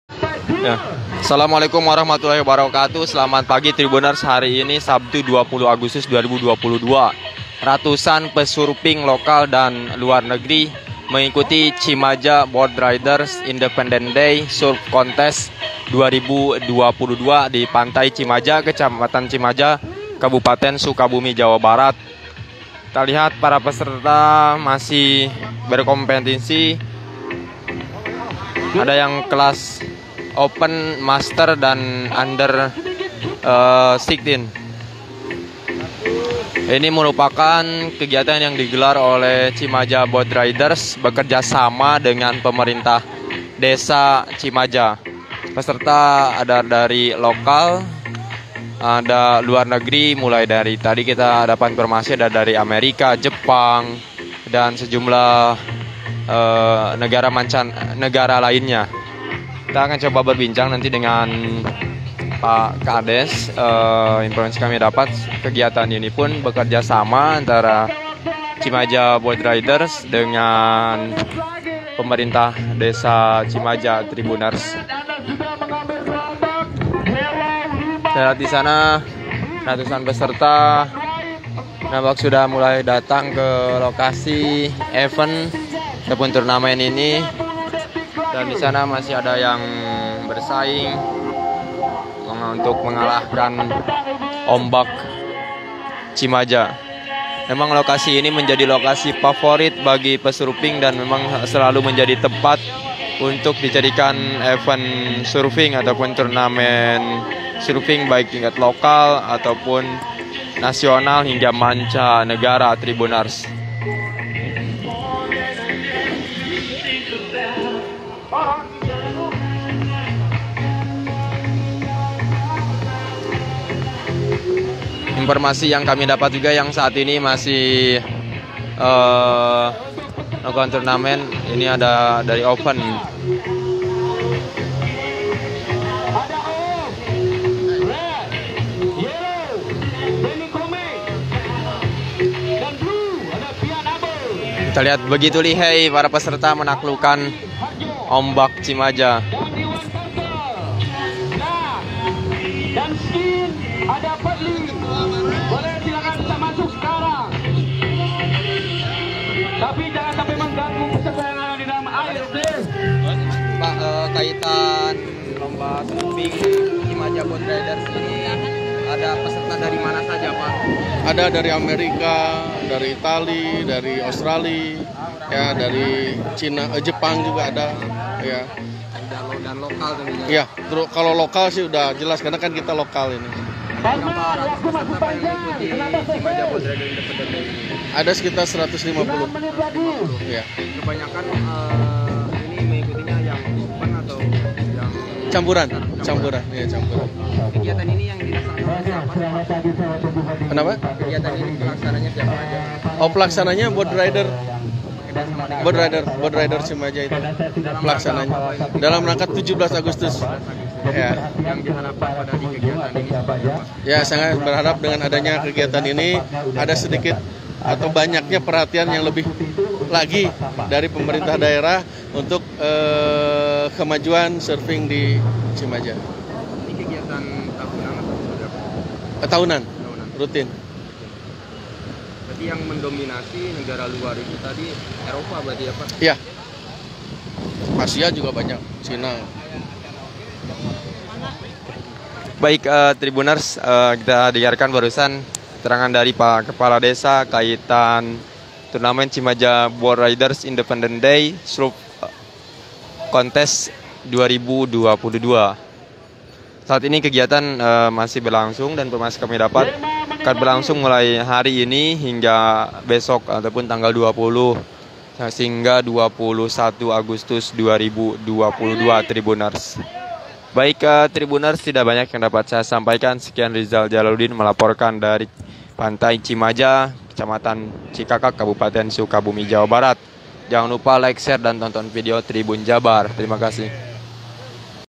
Ratusan Peselancar Lokal Hingga Mancanegara Ikuti Cimaja Boardriders Independence Day Surf Contest
TRIBUNCIREBON.COM - Ratusan peselancar lokal hingga luar negeri mengikuti kontes selancar hari kemerdekaan di pantai Cimaja, Desa Cimaja, Kecamatan Cikakak, Kabupaten Sukabumi, Jawa Barat, Sabtu (20/8/2022).

Kegiatan ini digelar oleh Cimaja Boardriders bekerja sama dengan Pemerintah Desa Cimaja. Ratusan peselancar ini adu kebolehan dalam bermain surfing di event Cimaja Boardriders Independence Day Surf Contest 2022.

Kepala Desa Cimaja, R Wahyu Cakra Ningrat mengatakan, peserta luar negeri ada dari Eropa dan Asia, kegiatan kontes selancar ini diikuti sekitar 150 peserta.

"Ada dari Amerika, dari Itali, Australi, China, Jepang juga ada, lokal-lokal sudah jelas, ada sekitar 150 peserta, ini kategori campuran. Pelaksananya Boardriders Cimaja dalam rangka 17 Agustus," ujarnya di pantai Cimaja.

Ia berharap kegiatan ini dapat menambah perhatian Pemerintah Daerah untuk mendukung kemajuan surfing di Cimaja. Diketahui, Cimaja merupakan lokasi favorit bagi warga mancanegara, terutama untuk bermain surfing.

"Saya berharap dengan adanya kegiatan ini ada sedikit banyaknya perhatian lagi dari Pemerintah Daerah untuk kemajuan surfing di Cimaja," jelasnya.